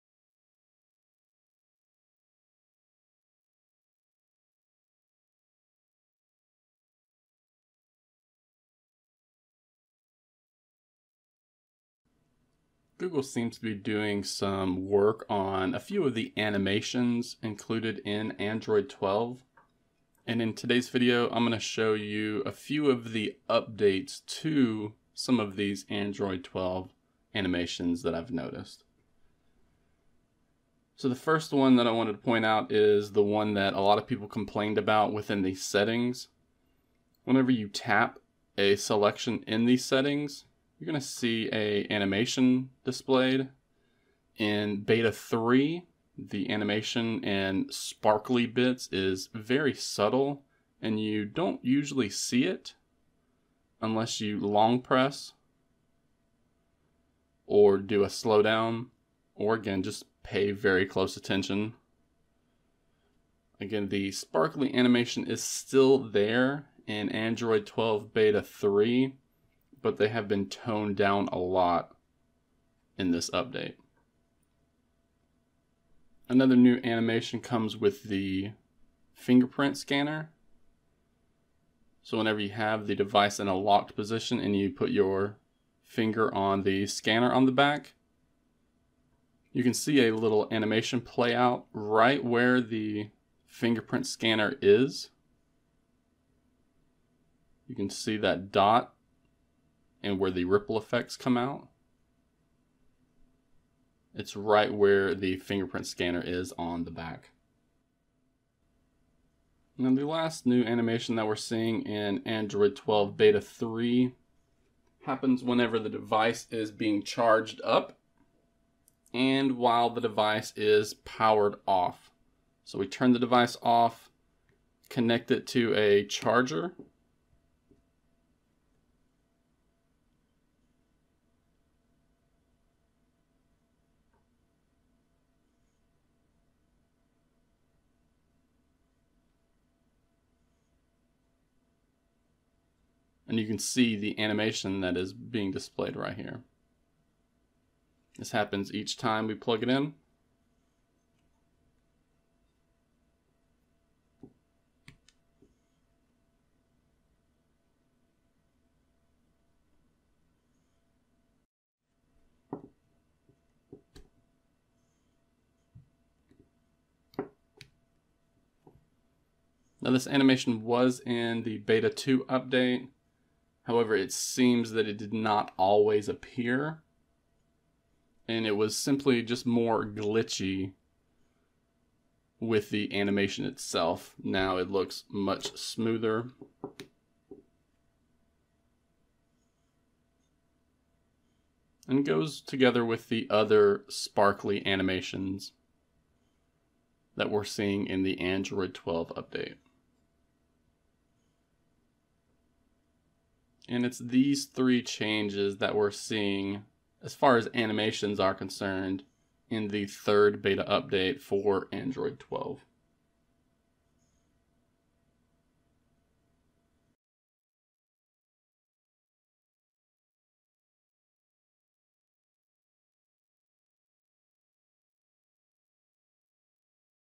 Android 12 Beta 3 Updates Animations Across the Operating System
Android 12 Beta 3 is now available and it comes with a number of new changes. Animation changes have been debated since the developer preview and this update adds more while also making some more subtle.

Many people have complaining about some of the animations that were added to the Android 12 update. Specifically, there were a lot of complaints about the animation that plays out when you tap a menu in the Settings application.

People just thought the update was buggy and was glitching out.

So in this update, Google seems to have made some changes to this animation while also improving and adding some extras as well.

In this video I also go over the new animation that plays out when you unlock the smartphone using the fingerprint scanner. This happens when I'm unlocking the Pixel 4a but likely happens with other Pixel phones too.

The last animation change was actually added in the beta 2 update but it was buggy and didn't always show up.

So I also take some time here to show the new animation that is displayed when the phone is charging (but it is powered off). It's another sparkly type animation that likely will likely be complained about as well.

None of these are groundbreaking features or changes and they just add a bit of flare to the user interface that Google is aiming for with the Android 12 update.

Full Tutorial
1. Intro [00:00]
2. Reduced tap/touch sparkle effect [00:31]
3. New Android 12 Fingerprint Scanner animation [01:15]
4. New Android 12 Powered Off Charging Animation [01:49]